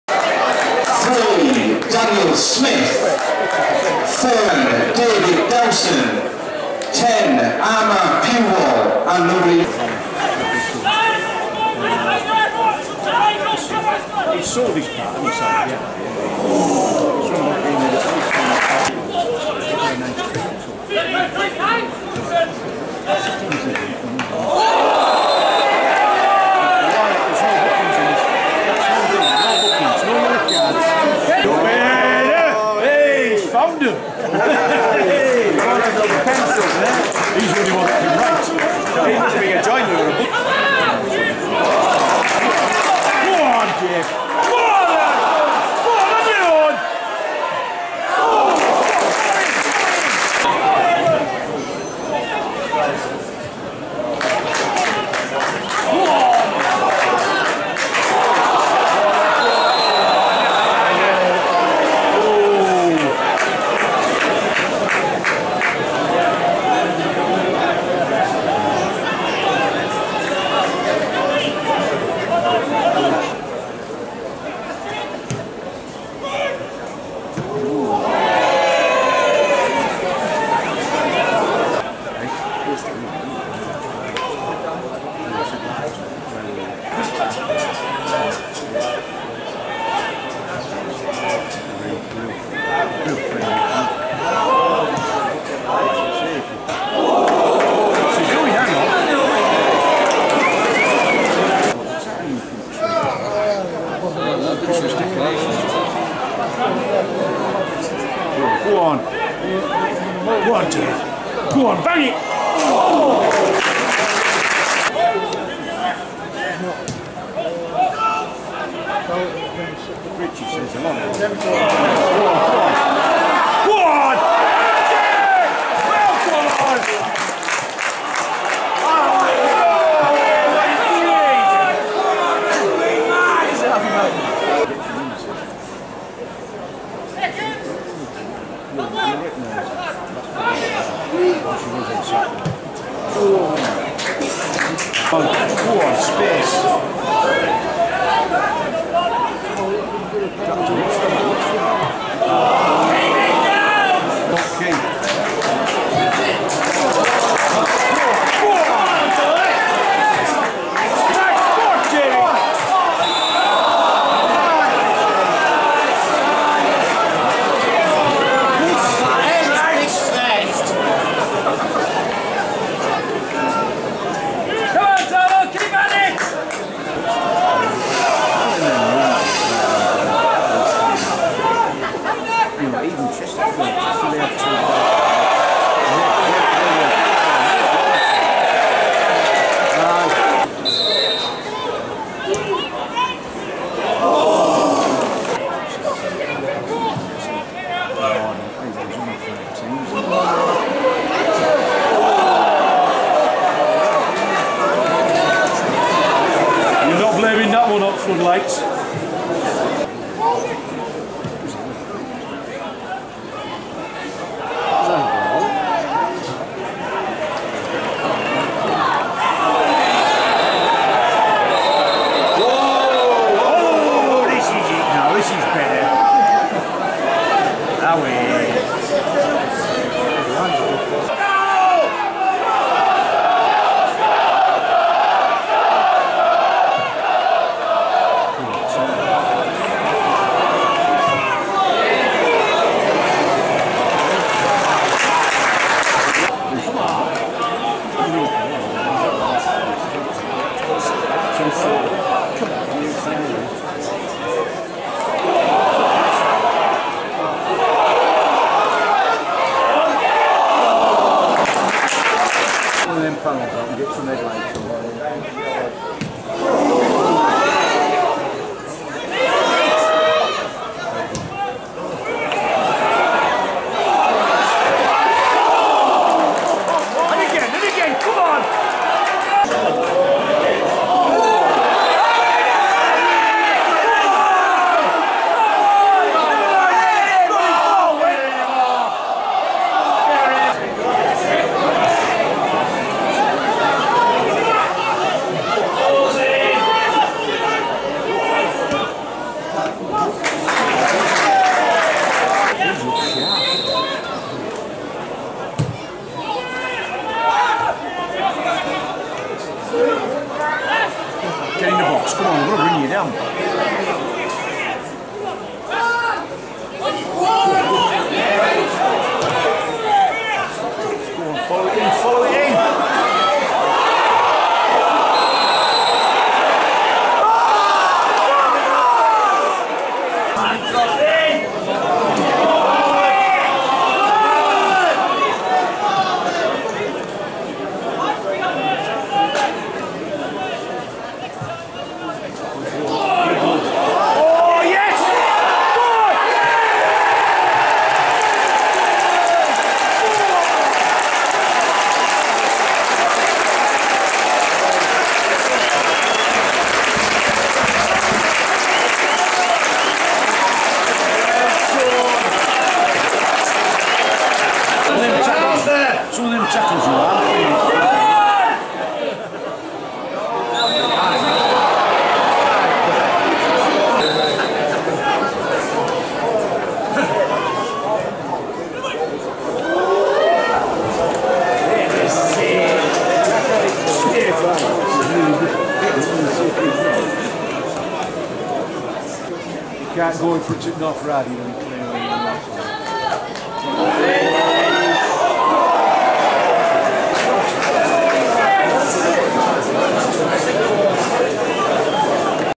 Darlington 1-0 Newton Aycliffe - Northern League Division 1 - 2012/13
Darlington managed to come away unscathed from a ferociously tight game at Heritage Park. A late winner from David Dowson saw all three points go to the home side and relieved the tension that was building around the ground.